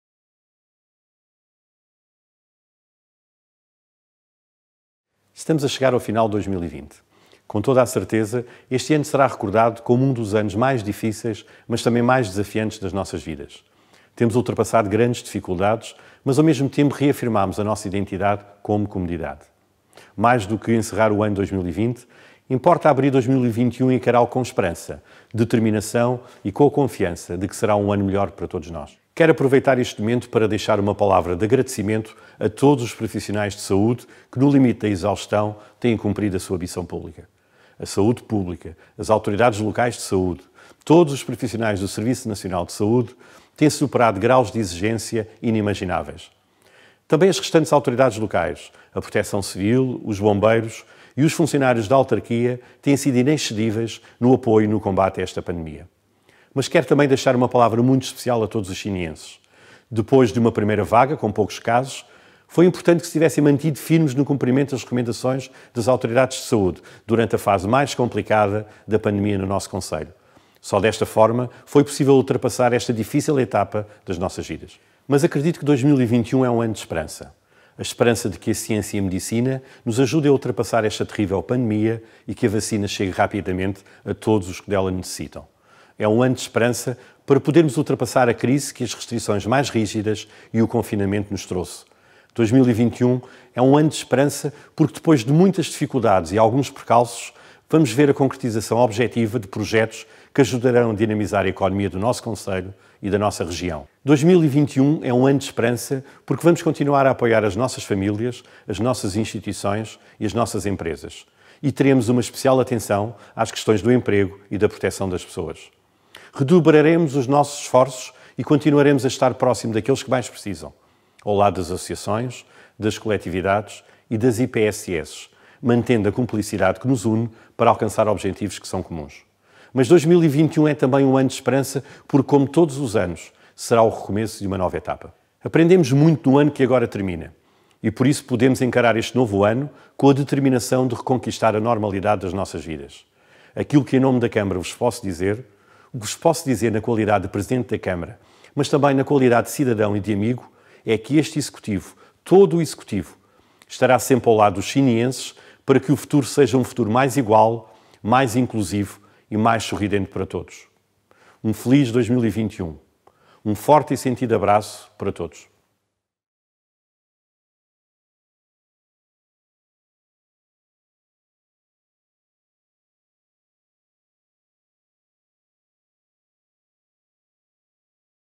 2021, Ano de Esperança | Mensagem de Ano Novo
Mensagem de Ano Novo pelo presidente da Câmara Municipal de Sines, Nuno Mascarenhas.